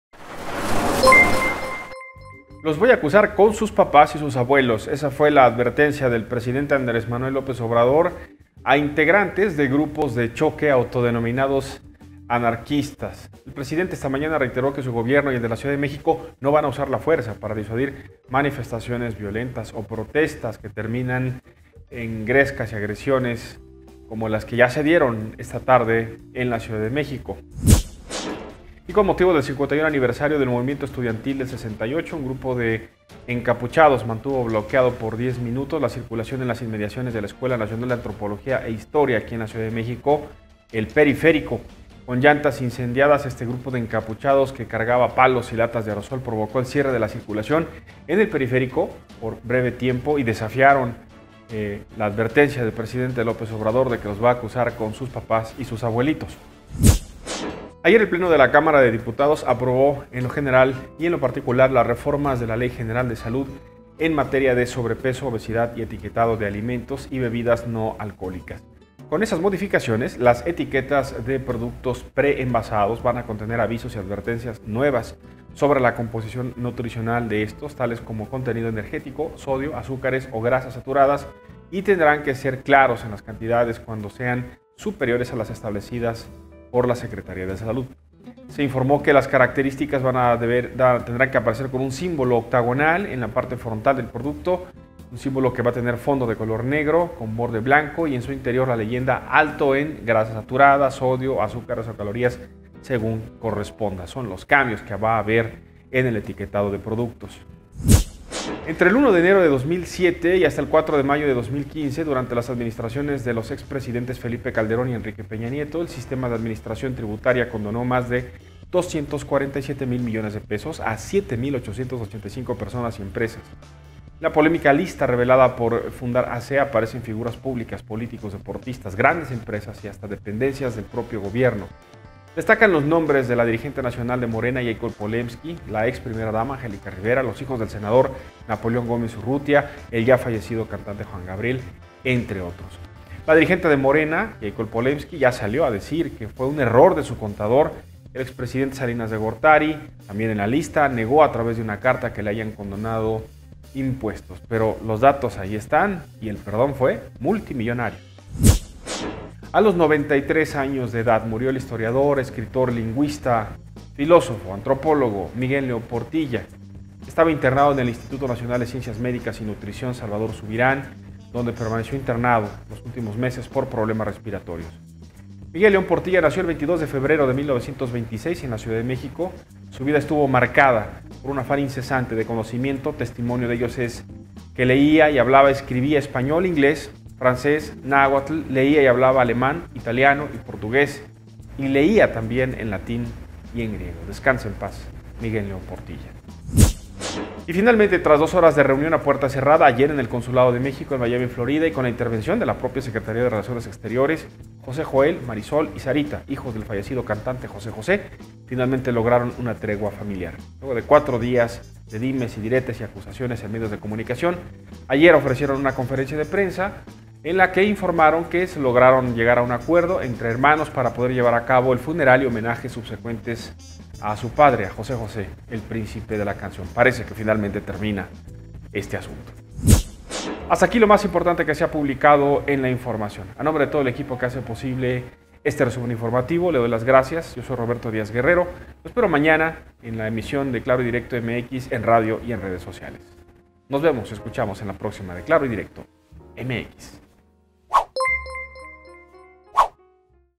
Resumen Informativo ClaroyDirectoMx || Miércoles 02 de octubre 2019
- Los voy a acusar con sus papás
- Encapuchados desafían a AMLO
- Aprobado el nuevo
- Dan a conocer beneficiarios de pago de impuestos
- Murió Miguel León Portilla
- Logran acuerdo hijos de JoséJosé